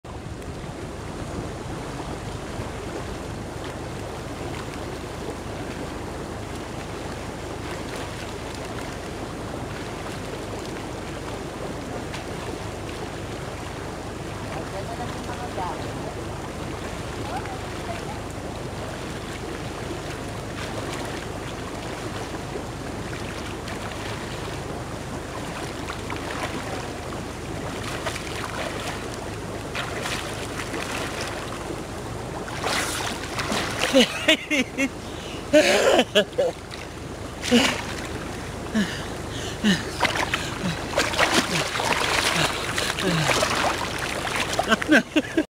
Breast stroke
One lap of 25 meters in the swimming pool of Casitas De Victoria in Nasugbu, Batangas